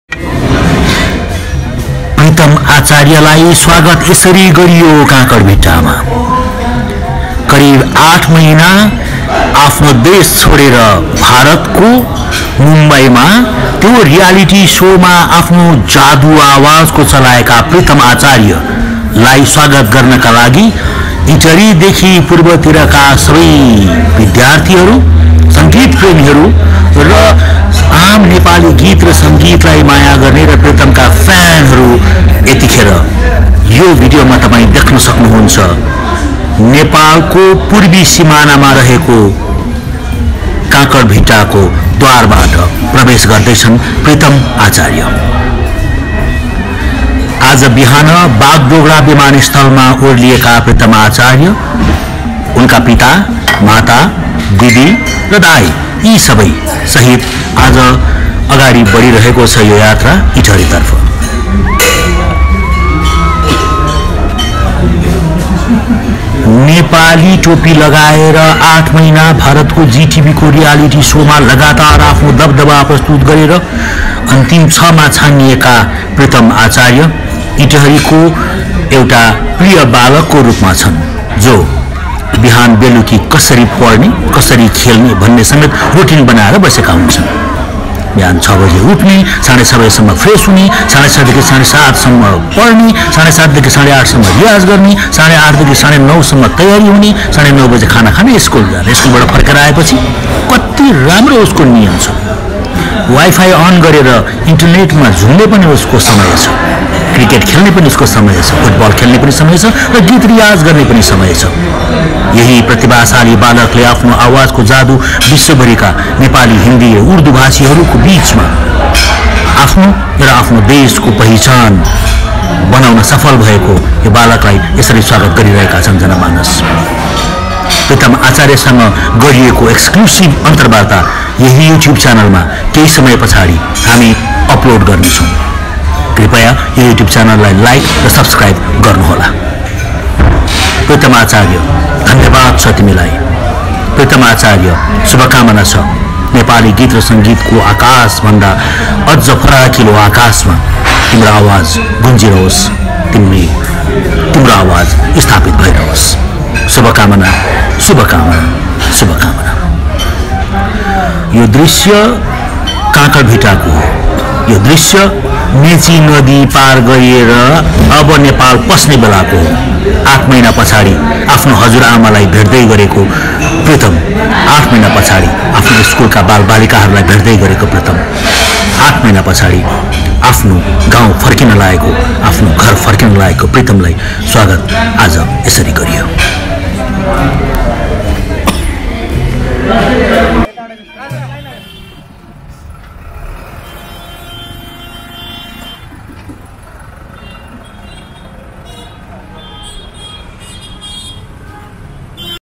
Welcoming Pritam in Nepal ! प्रीतम आचार्यलाई स्वागत नेपालमा
इटहरी नै उर्ल्यो प्रितमको आचार्यको स्वागतमा !